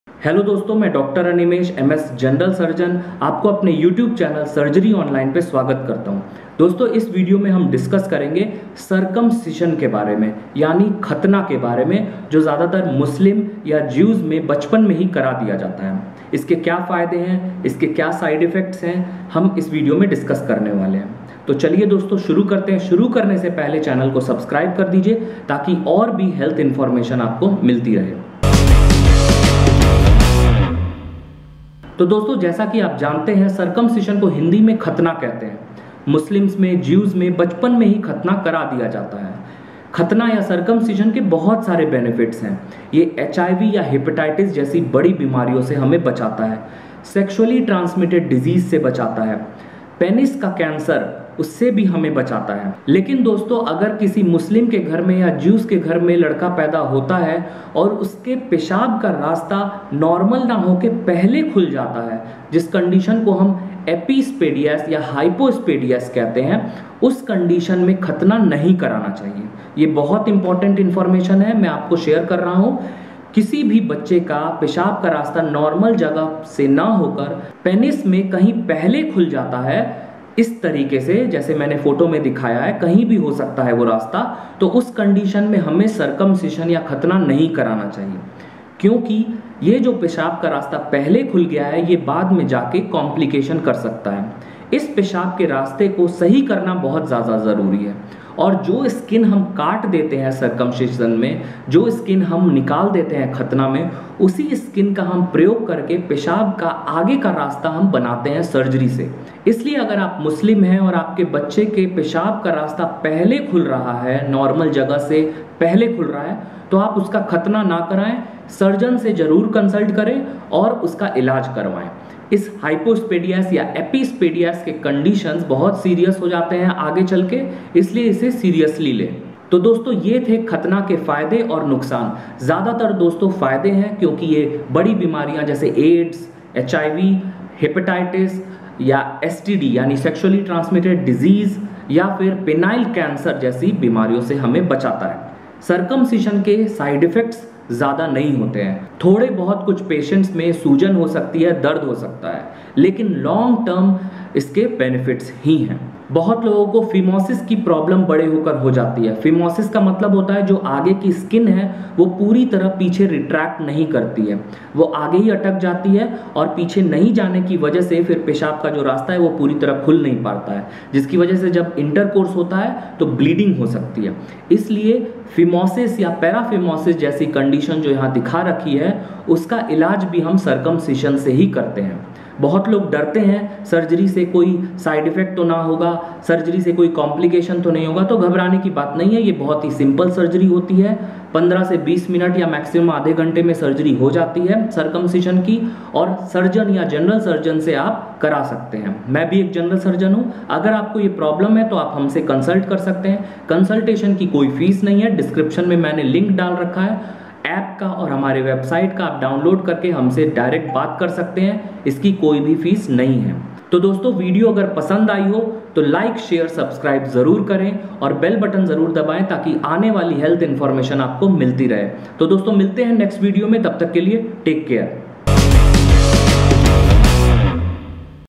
Circumcision क्या होता है? Benefits & side effects? कब कराना च‍ाहिए और कब नहीं? by Dr Animesh MS
for more details.
In this video, i have tried to explain about the surgery - Circumcision, when to do the surgery and when not to do?